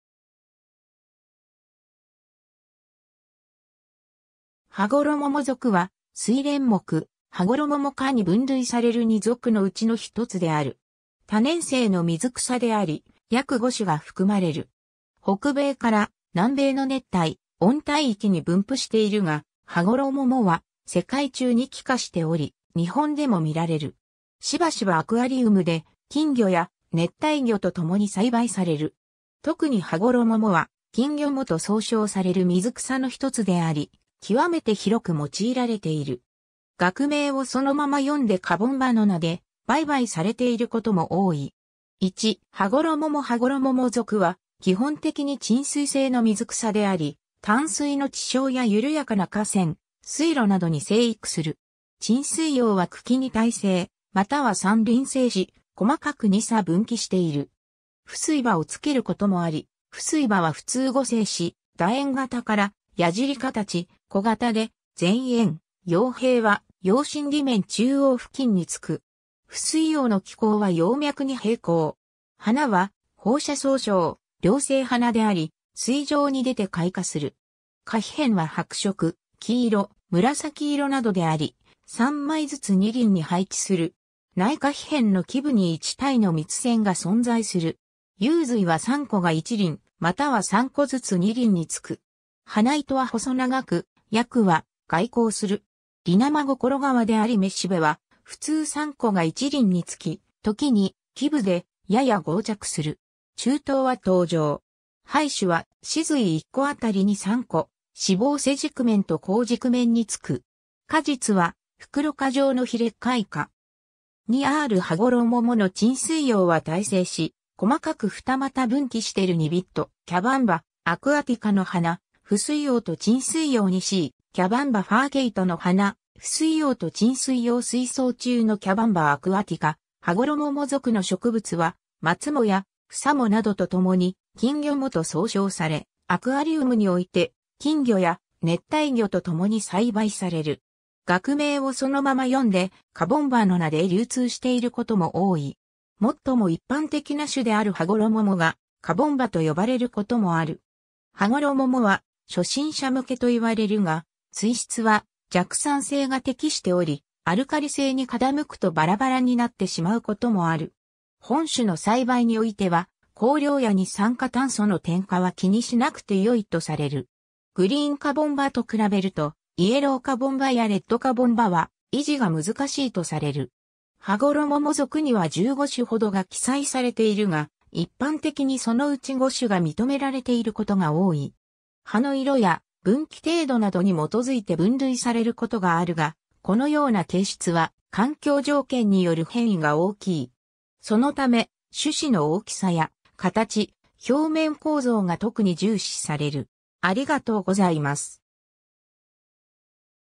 ハゴロモモ属
ハゴロモモ属 (学名: Cabomba) は、スイレン目ハゴロモモ科 (ジュンサイ科ともよばれ、またスイレン科に含められることもあった) に分類される2属のうちの1つである (もう1つはジュンサイ属)。
多年生の水草であり、約5種が含まれる。
北米から南米の熱帯〜温帯域に分布しているが、ハゴロモモ (フサジュンサイ) は世界中に帰化しており、日本でも見られる。
 しばしばアクアリウムで金魚や熱帯魚とともに栽培される。
特にハゴロモモは金魚藻と総称される水草の1つであり、極めて広く用いられている。
学名をそのまま読んでカボンバの名で売買されていることも多い。
 1. ハゴロモモ (右側は雌蕊群) ハゴロモモ属は、基本的に沈水性の水草であり、淡水の池沼や緩やかな河川、水路などに生育する。
沈水葉 (水中葉) は茎に対生または3輪生し、細かく二叉分岐している (図1, 2a)。
浮水葉 (水上葉) をつけることもあり、浮水葉はふつう互生し、楕円形からやじり形、小型で全縁、葉柄は葉身裏面中央付近につく (楯状) (図1, 2b, c)。
浮水葉の気孔は葉脈に平行。
 花は放射相称、両性花であり、水上にでて開花する (図1, 2b, c)。
花被片は白色、黄色、紫色などであり、3枚ずつ2輪に配置する (図2b, c)。
内花被片の基部に1対の蜜腺が存在する。
雄蕊は3個が1輪、または3個ずつ2輪につく。
花糸は細長く、葯は外向する。
離生心皮であり雌蕊はふつう3個が1輪につき、ときに基部でやや合着する。
柱頭は頭状。
胚珠は雌蕊1個あたり2–3個、子房背軸面と向軸面につく。
果実は袋果状の非裂開果。
 2a. ハゴロモモの沈水葉は対生し、細かく二又分岐してる 2b. Cabomba aquatica の花、浮水葉と沈水葉 2c. Cabomba furcata の花、浮水葉と沈水葉 水槽中の Cabomba aquatica ハゴロモモ属の植物は、マツモ (マツモ科) やフサモ (アリノトウグサ科) などとともに 金魚藻（きんぎょも）と総称され、アクアリウムにおいて金魚や熱帯魚とともに栽培される。
学名をそのまま読んで「カボンバ」の名で流通していることも多い。
もっとも一般的な種であるハゴロモモ (グリーンカボンバ) がカボンバとよばれることもある)。
 ハゴロモモ (グリーンカボンバ) は初心者向けといわれるが、水質は弱酸性が適しており、アルカリ性に傾くとバラバラになってしまうこともある。
本種の栽培においては光量や二酸化炭素の添加は気にしなくて良いとされる。
グリーンカボンバとくらべると、イエローカボンバやレッドカボンバは維持が難しいとされる。
 ハゴロモモ属には15種ほどが記載されているが、一般的にそのうち5種が認められていることが多い (下表)。
葉の色や分岐程度などに基づいて分類されることがあるが、このような形質は環境条件による変異が大きい。
そのため、種子の大きさや形、表面構造が特に重視される。